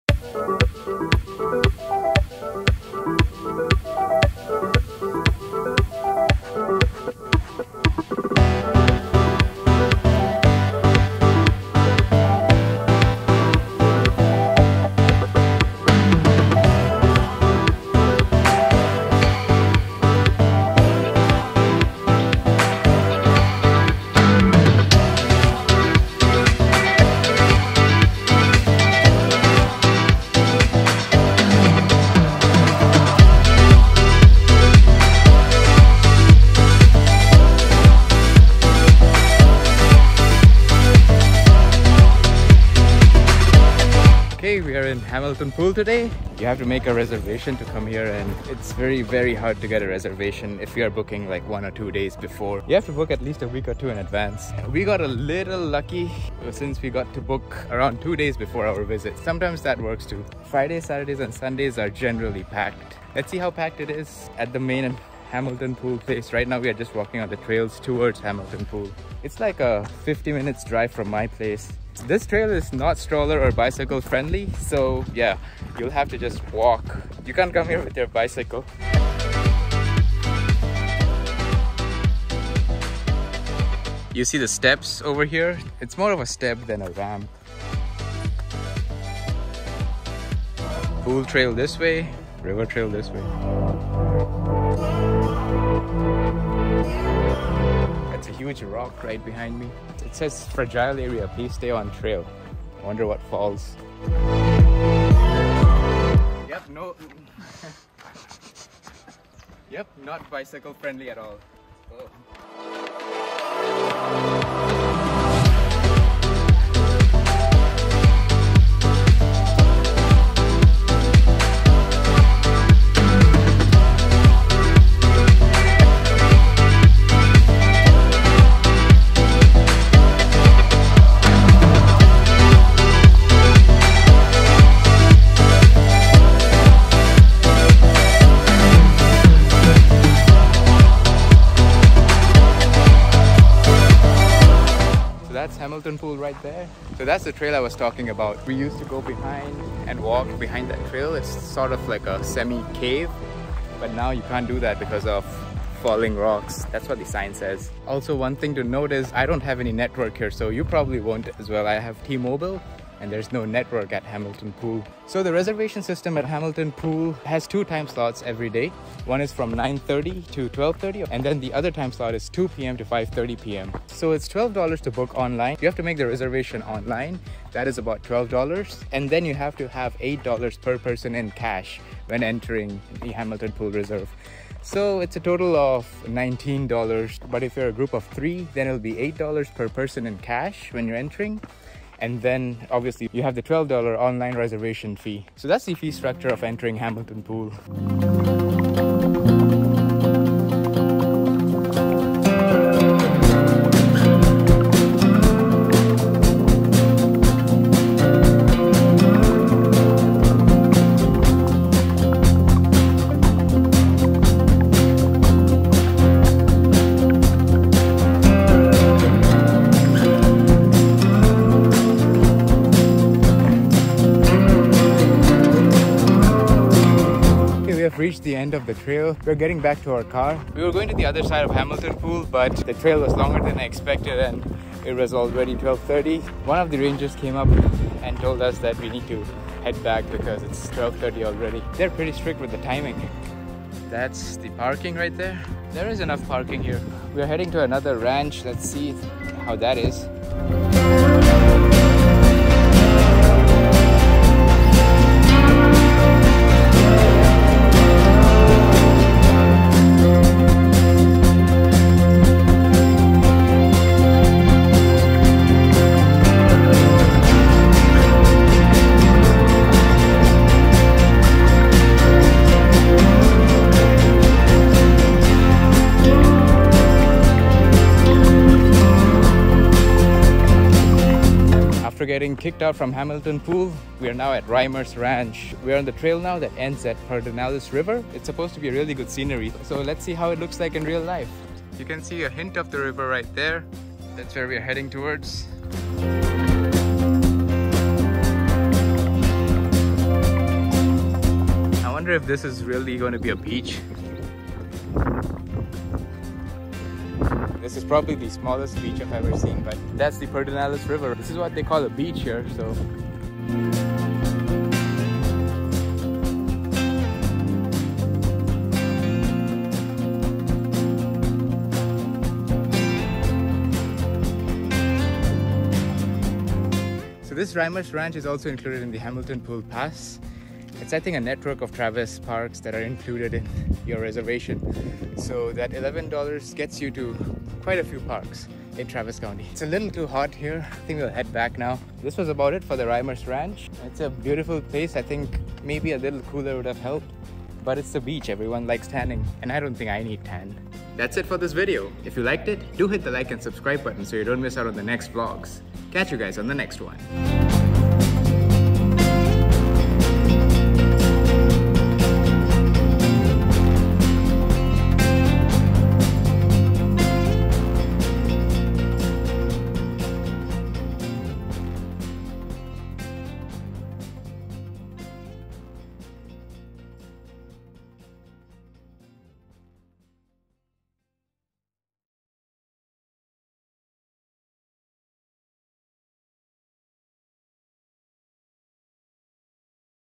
Hamilton Pool | Quick Trip | Austin Vlog
In this vlog, we head to Hamilton Pool near Austin! Hamilton Pool Preserve is a recreational destination located in the Texas Hill Country west of Austin. Reservations are required to visit, and it is open for hiking and sight-seeing. Hamilton Pool is a natural pool that was created when the dome of an underground river collapsed due to massive erosion thousands of years ago. The pool is located about 23 miles (37 km) west of Austin, Texas off Highway 71. Since the 1960s, Hamilton Pool has been a popular summer swimming spot for Austin visitors and residents, but swimming is no longer allowed due to issues with falling rocks and other signs of instability of the large grotto. Hamilton Pool Preserve consists of 232 acres (0.94 km2) of protected natural habitat featuring a jade green pool into which a 50-foot (15 m) waterfall flows with water temperatures reaching 50 degrees Fahrenheit.